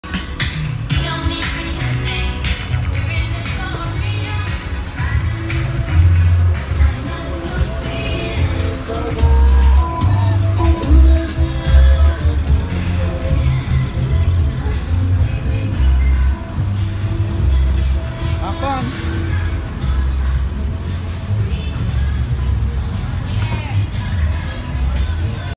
Lowriders in Santa Ana
Ilustrious Barbque , hustlers life cc summer breeze and el padrino